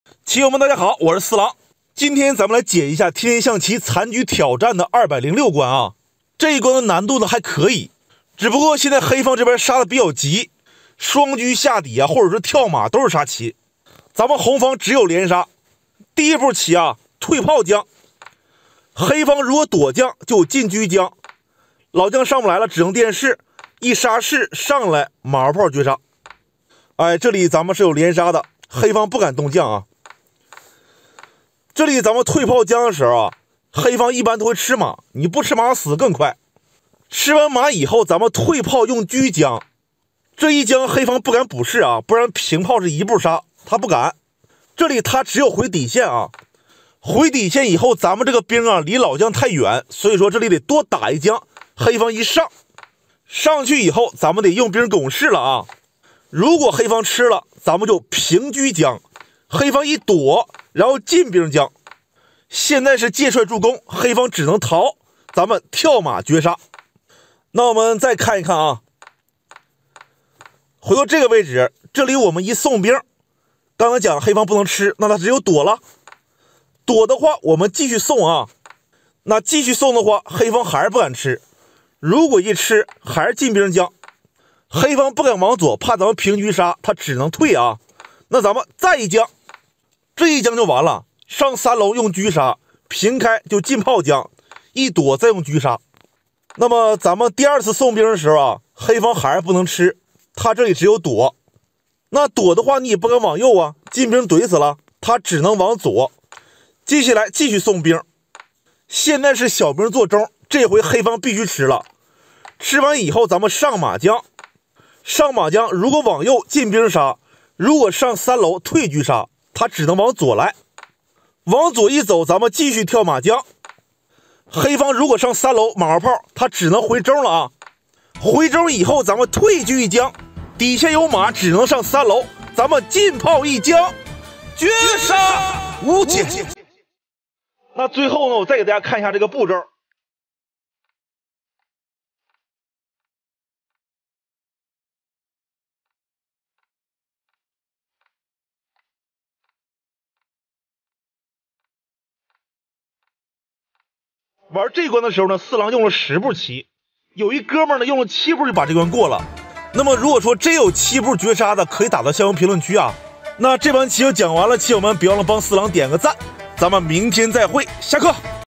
残局挑战，车马炮兵绝妙连杀！三维立体的攻势！激情解说【四郎讲棋】
象棋 象棋对战 残局挑战 四郎讲棋 请注意，本视频完全符合YouTube社区准则，没有任何违规行为。